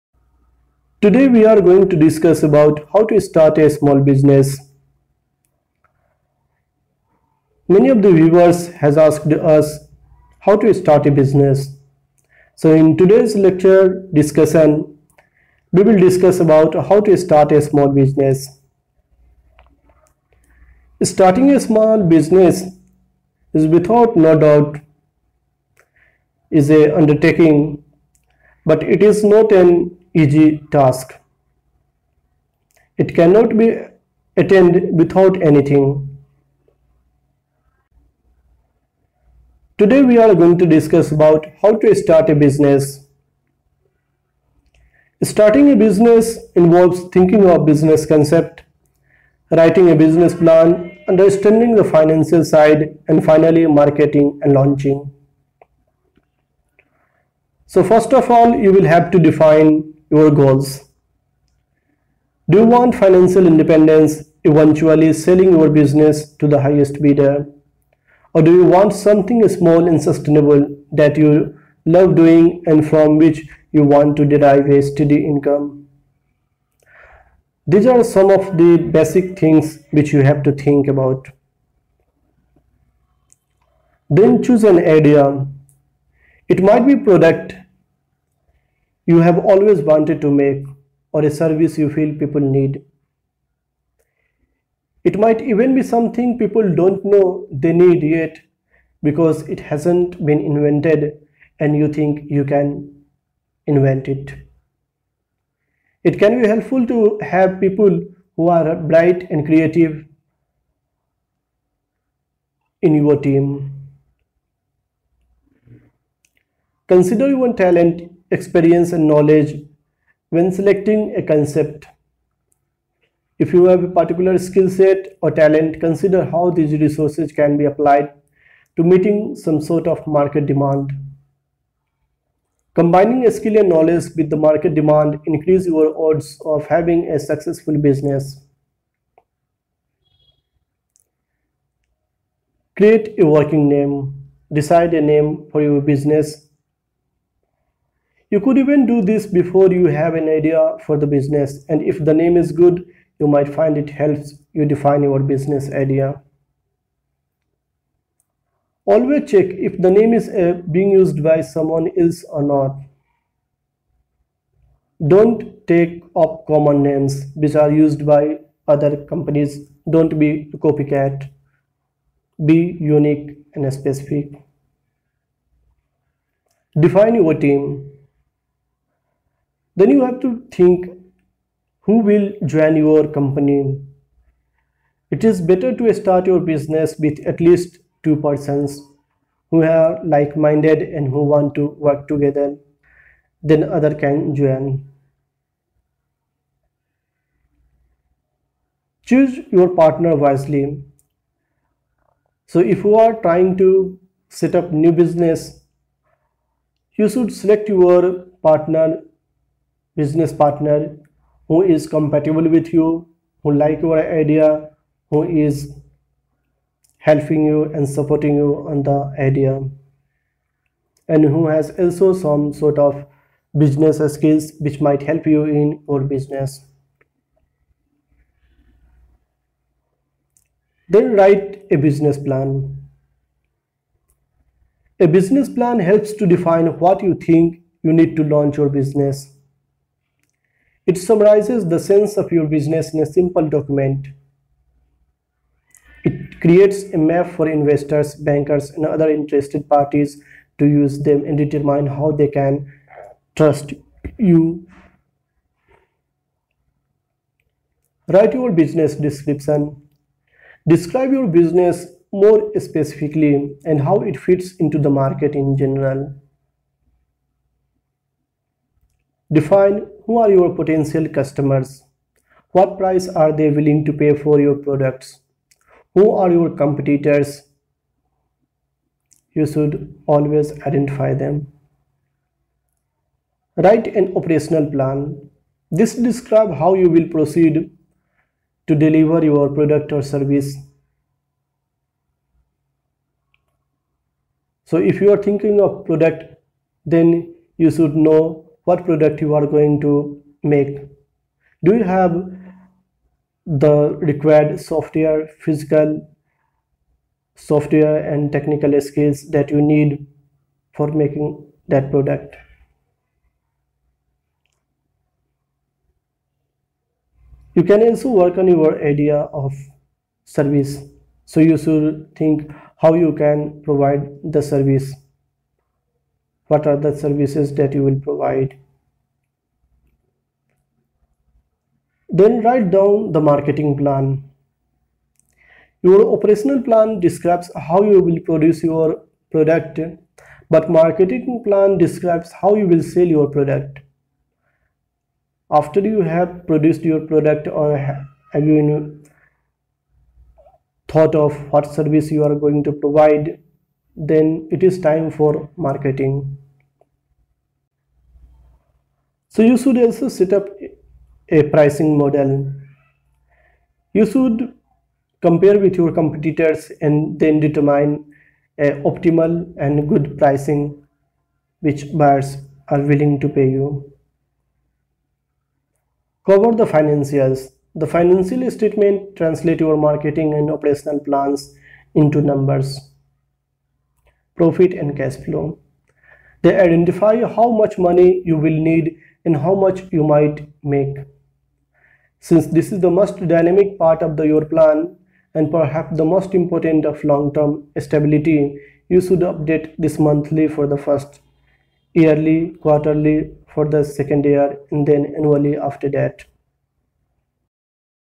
How to Start a Business without Money
You need a plan in order to make your business idea a reality. A business plan is a blueprint that will guide your business from the start-up phase through establishment and eventually business growth, and it is a must-have for all new businesses.

The good news is that there are different types of business plans for different types of businesses.

If you intend to seek financial support from an investor or financial institution, a traditional business plan is a must. This type of business plan is generally long and thorough and has a common set of sections that investors and banks look for when they are validating your idea.

If you don't anticipate seeking financial support, a simple one-page business plan can give you clarity about what you hope to achieve and how you plan to do it. In fact, you can even create a working business plan on the back of a napkin, and improve it over time. Some kind of plan in writing is always better than nothing.

Step 3: Plan Your Finances
Starting a small business doesn't have to require a lot of money, but it will involve some initial investment as well as the ability to cover ongoing expenses before you are turning a profit.